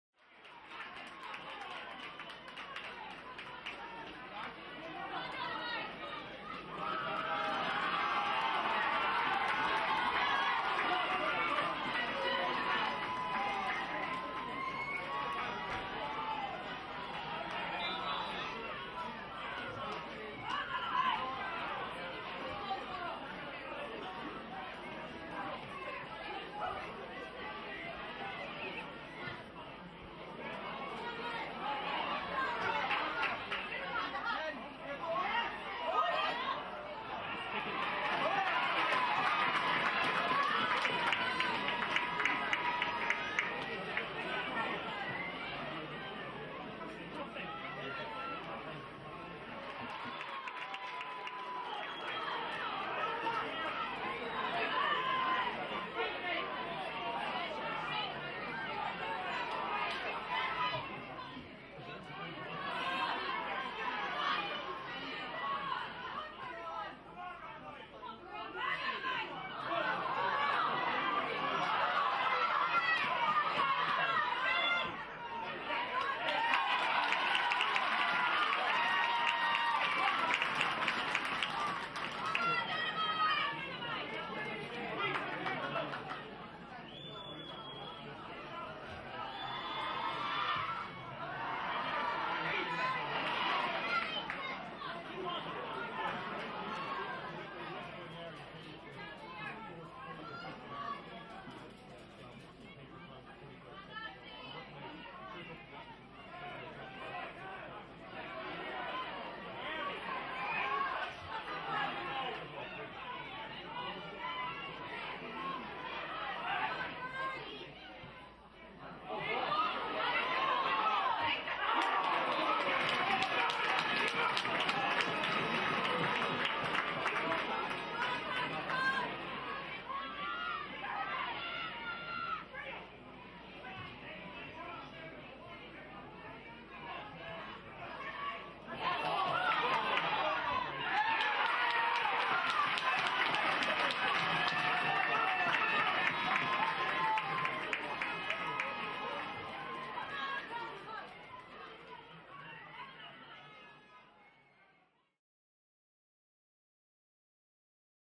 Donaghmoyne's Winning Move
See new All Ireland champions Donaghmoyne make a matchwinning move by opening up a five-point lead early in the second-half of the Final against Cornacon.

Player of the Match Cora Courtney was among the scorers as the Monaghan team extended their half-time advantage.

More highlights and interviews will follow, courtesy of LGFA.